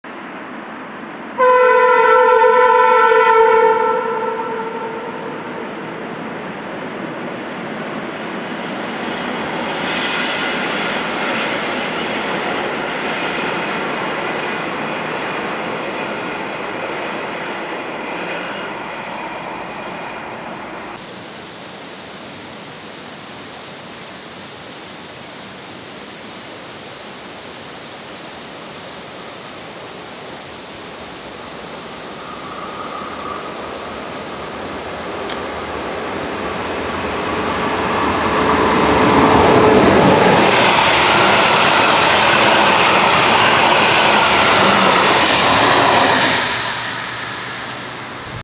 Tåg vid Risbohult-Kvarnberget
Två tåg vid ungefär samma ställe mellan Härryda och Hindås. Det hade regnat strax innan det senare tåget kom, därav vattensprutet från strömavtagaren.